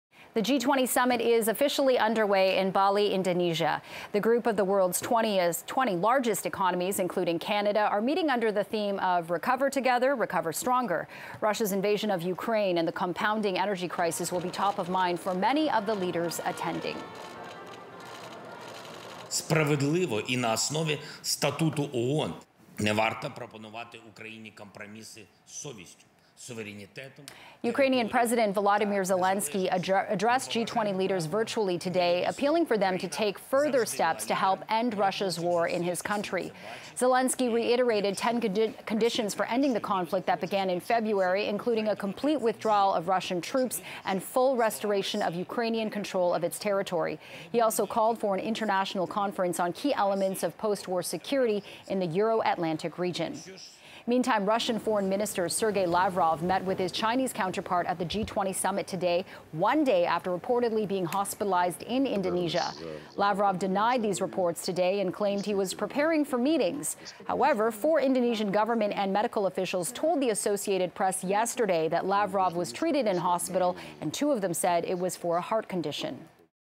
Sergey Lavrov denies reports that he was hospitalized in Indonesia
Russian Foreign Minister Sergey Lavrov met with his Chinese counterpart at the G20 Summit today; one day after reportedly being hospitalized in Indonesia.